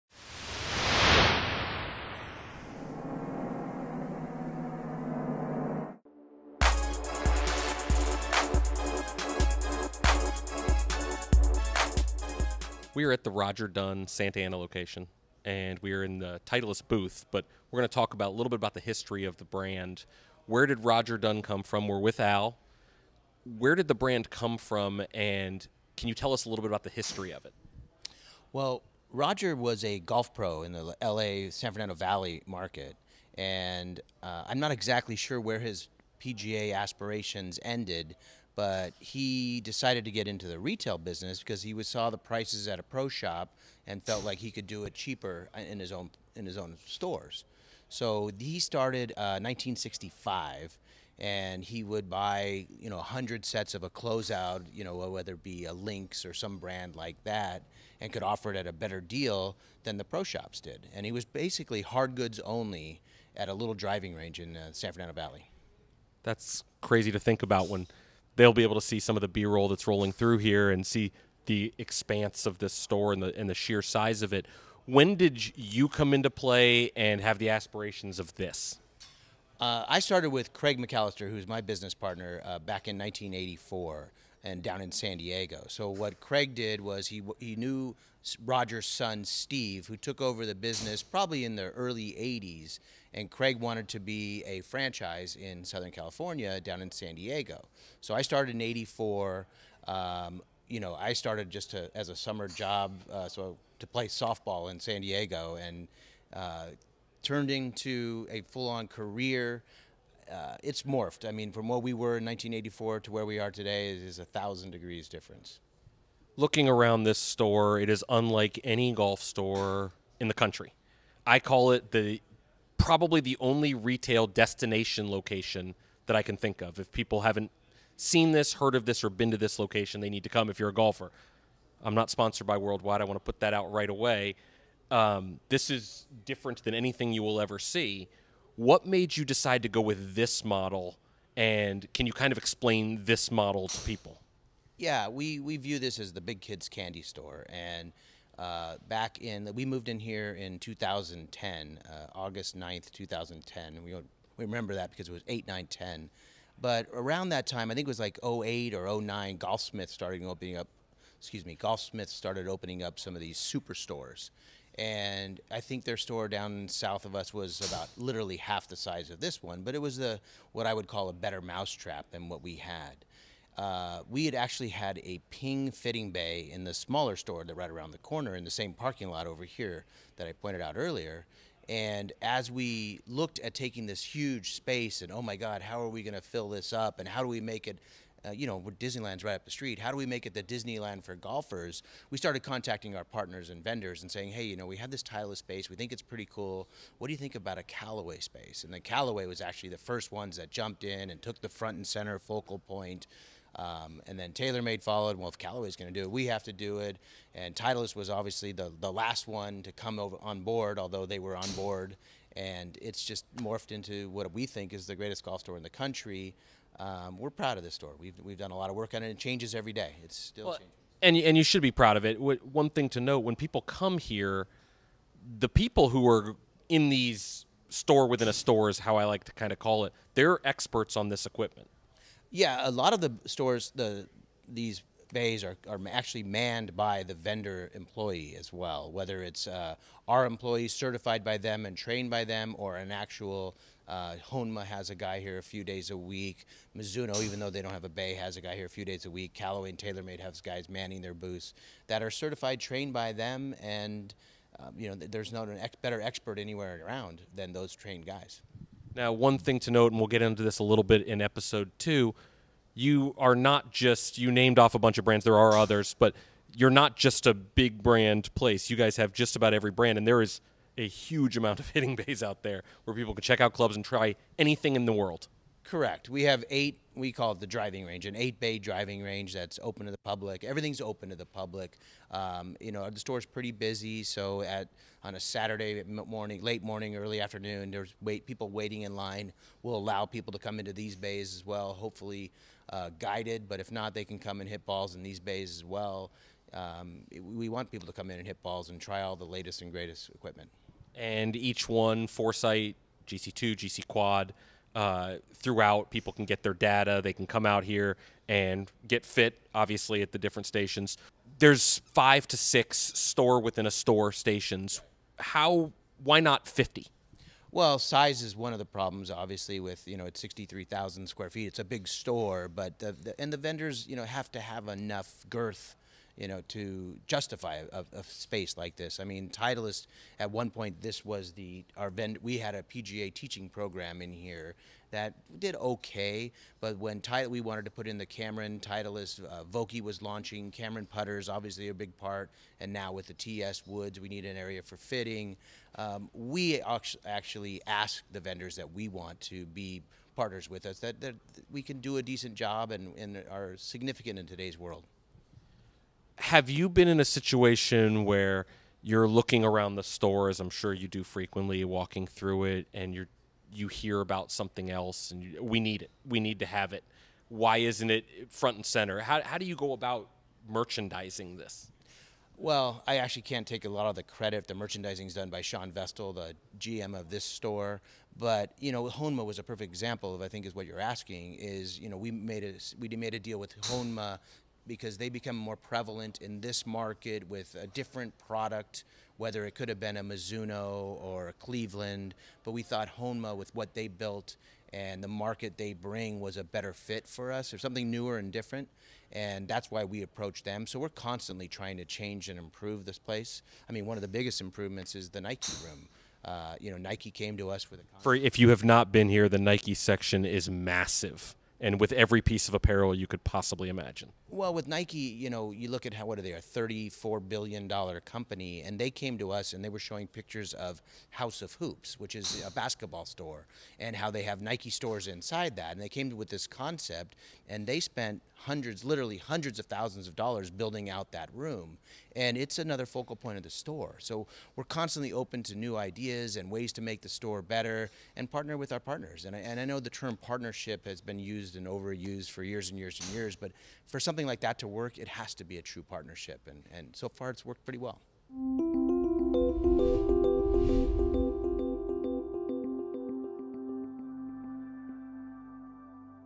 Behind the Scenes with Worldwide Golf
When you enter this golf mecca, you realize that you are not in just an ordinary golf store, but rather something extraordinary.  We got a chance to sit down with Al Morris, CEO of Worldwide Golf and discuss the entire history of the brand and where they are headed in the future.

In Episode 1 (of 3), we discuss the best golf store in the country, Roger Dunn Santa Ana location and why it's considered that by most.  What makes it unique?  How do they maintain it?

The places to find us for tons of contests and events.